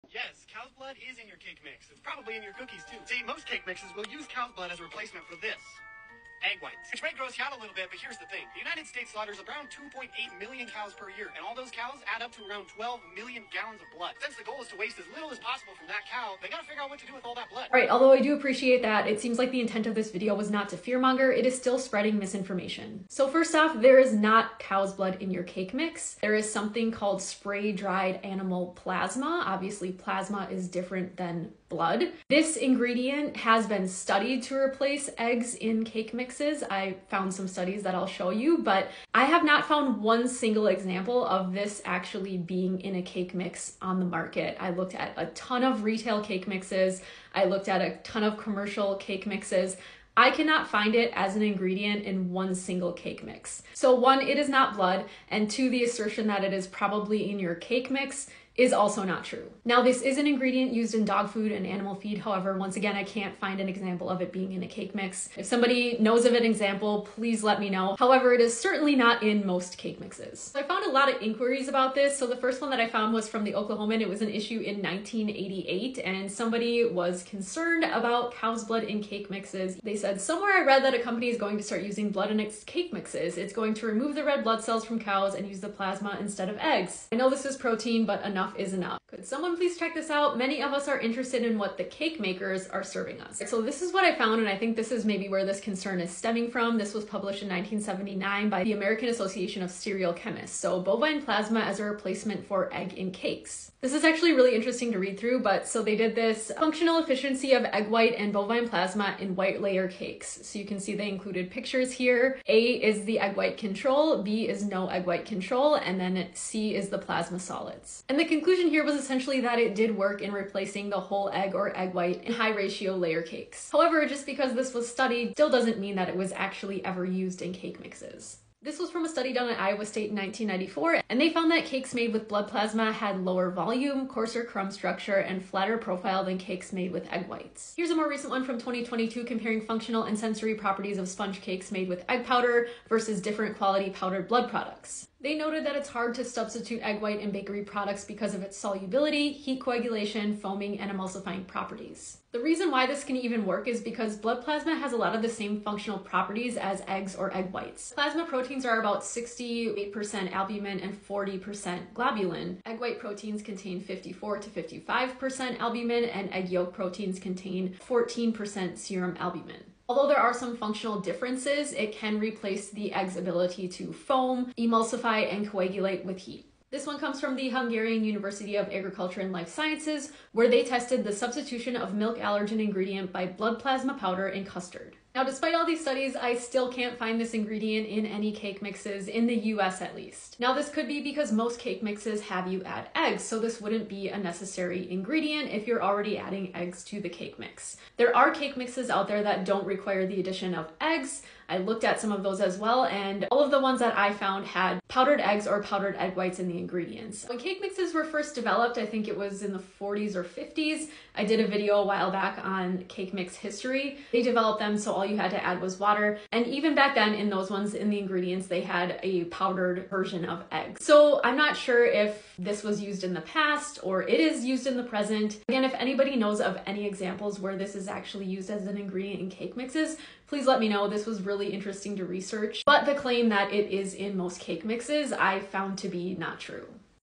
Cows Blood In Cake Mix?
Fact Check: Is there really cow blood in your cake mix?